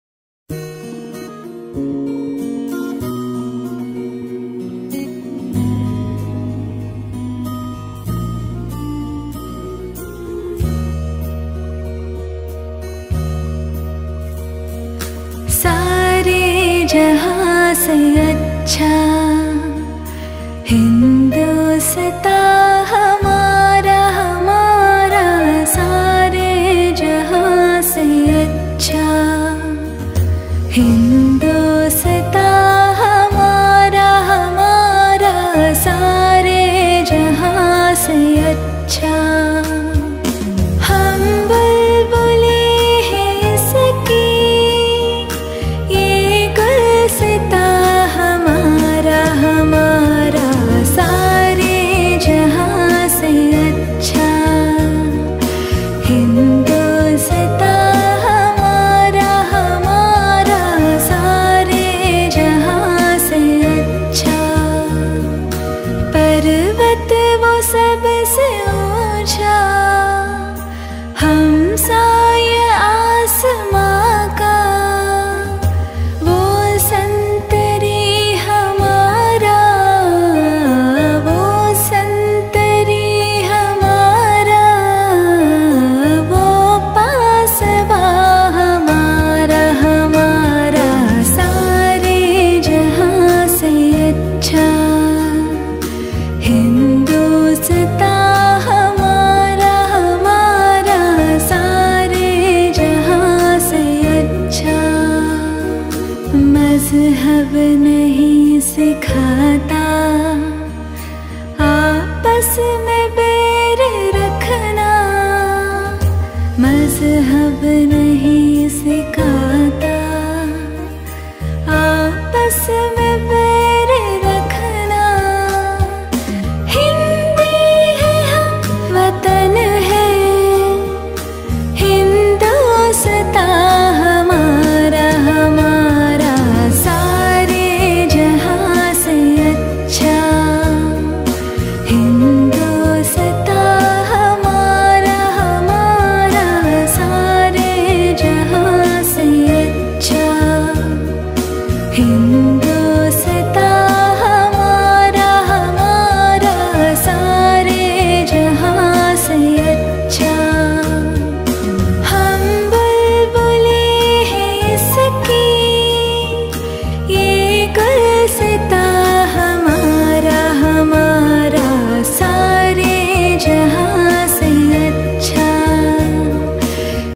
Our Heartfelt Gratitude to our great Scientists of ISRO for creating history of Chandrayaan - 3
By Growing Soul Kidz Gurukul School, Mathura